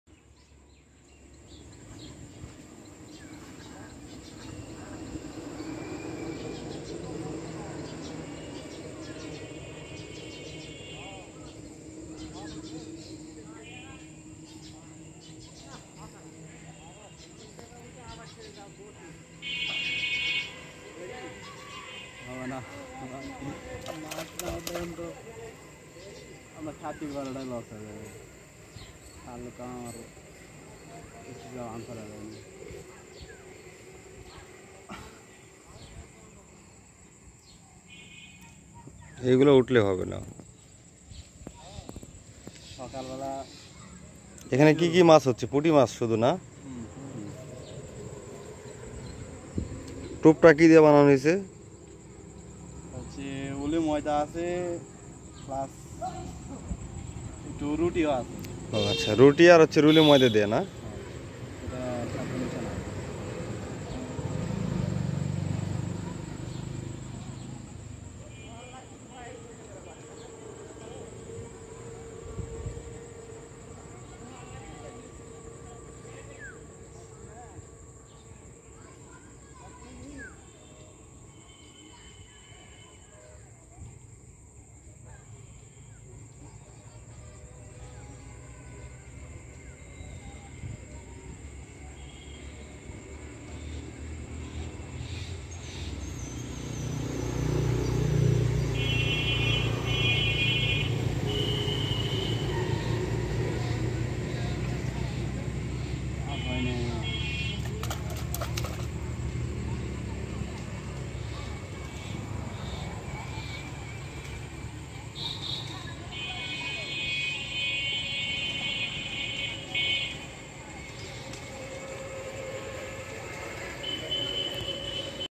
Best Fishing Video | Fishing with Hook | Catching Catfish by Hook
Hi Friends
Welcome to another fishing video fishing with hook.In this video man catching catfish by Hook.This is very popular Fishing in our country.I hope you will like this video.